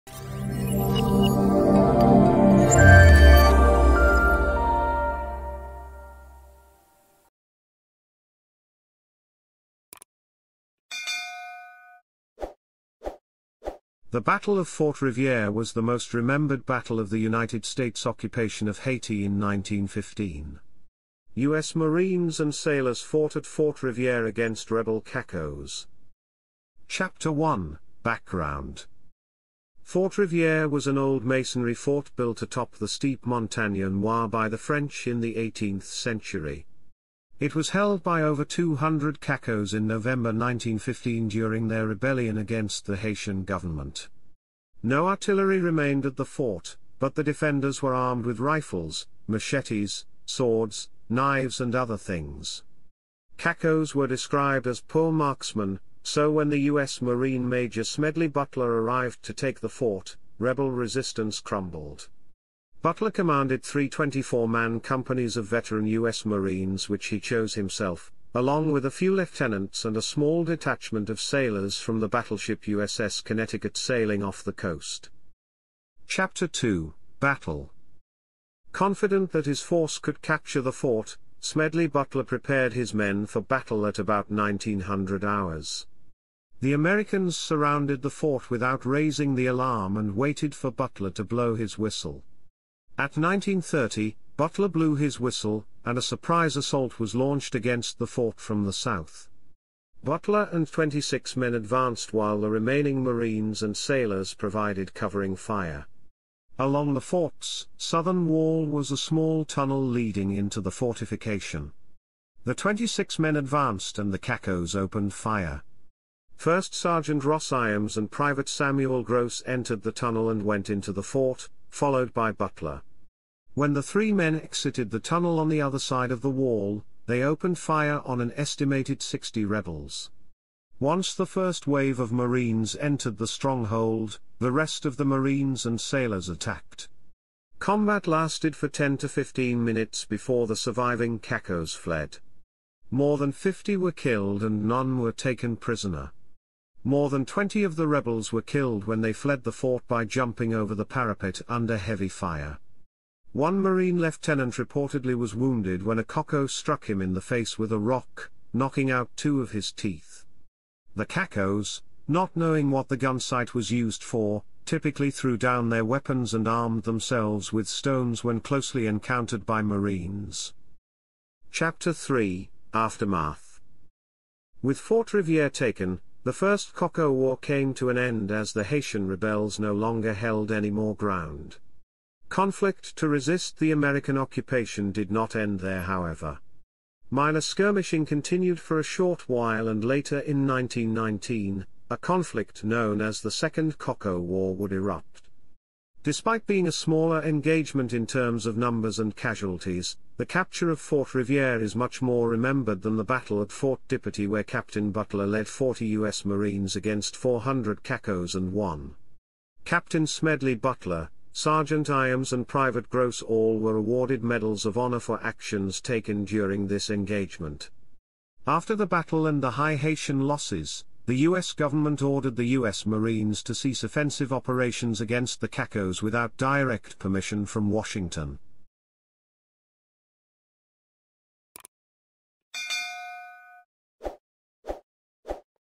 Battle of Fort Rivière - Wikipedia article
Video summary:
The Battle of Fort Rivière was the most remembered battle of the United States occupation of Haiti in 1915. U.S Marines and sailors fought at Fort Rivière against rebel Cacos.